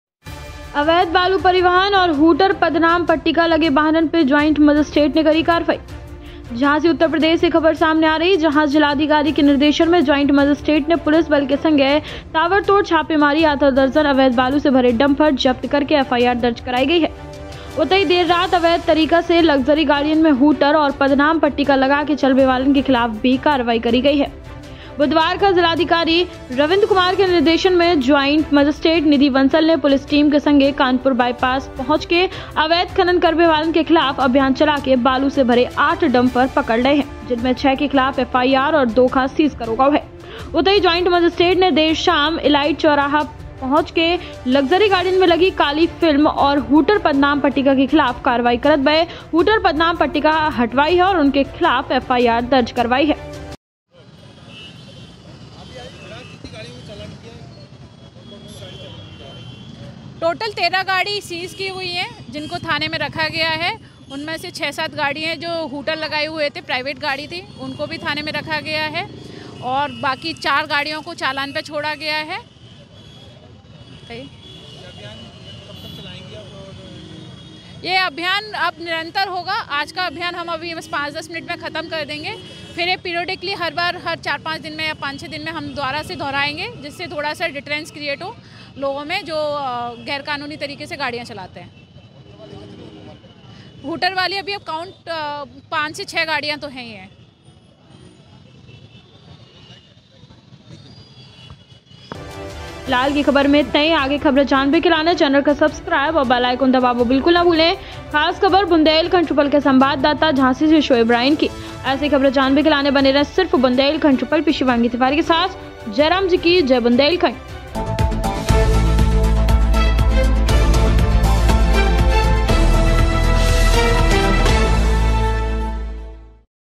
Jhansi: अवैध बालू परिवहन और हूटर पदनाम पट्टिका लगे वाहनों पर जॉइंट मजिस्ट्रेट ने की कार्यवाही
अवैध वालू परिवहन और हूटर पदनाम पट्टिका लगे वाहनों पर ज्वाइंट मजिस्ट्रेट ने की कार्यवाही
झांसी। जिलाधिकारी के निर्देशन में ज्वाइंट मजिस्ट्रेट ने पुलिस बल के साथ आज ताबड़तोड़ छापेमारी आधा दर्जन अवैध वालू से भरे डंफर जब्त कर एफ आई आर दर्ज कराई गई। वही देर रात अवैध तरीके से लग्जरी गाड़ियों में हूटर और पदनाम पट्टिका लगाकर चलने वालों के खिलाफ कार्यवाही की गई।
bundelkhand news mp,
bundelkhand samachar,
bundelkhandi,
bundelkhandi language,
jhansi news,